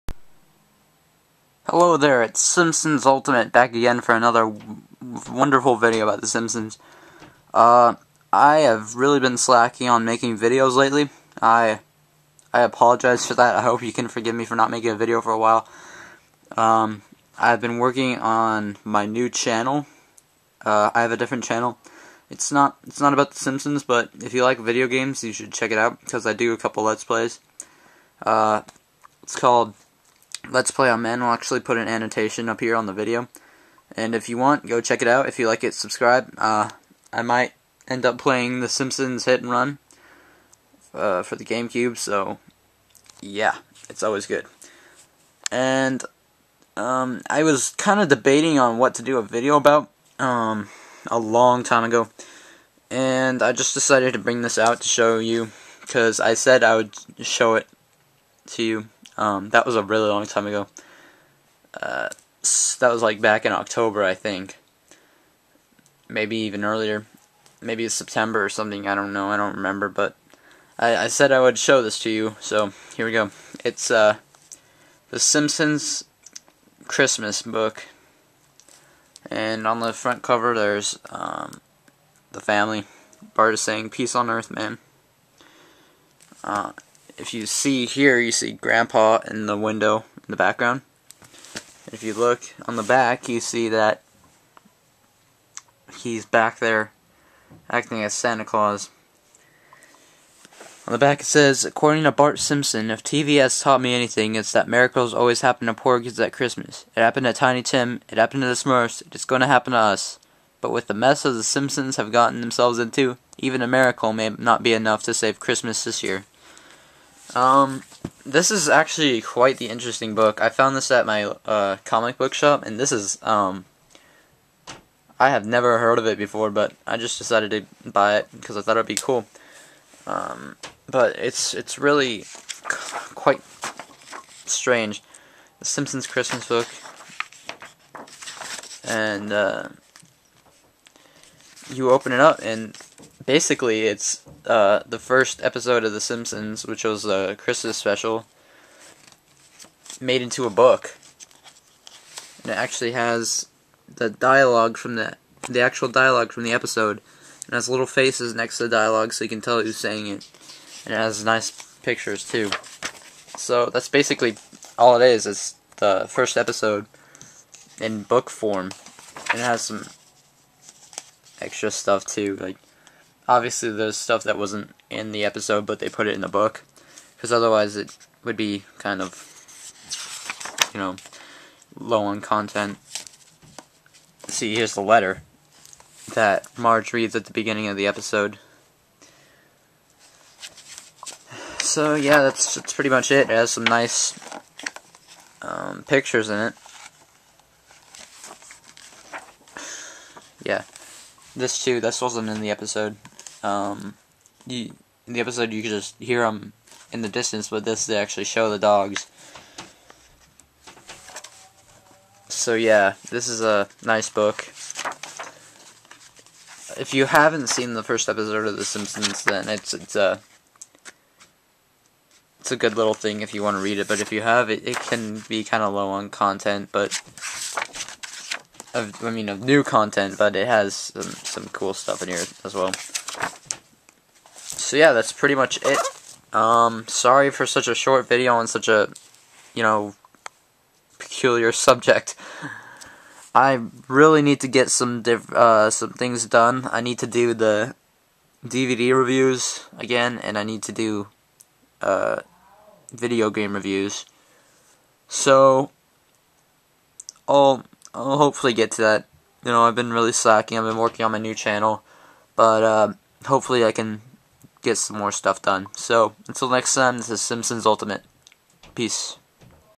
The Simpsons X-Mas Book
I review The Simpsons X-Mas Book.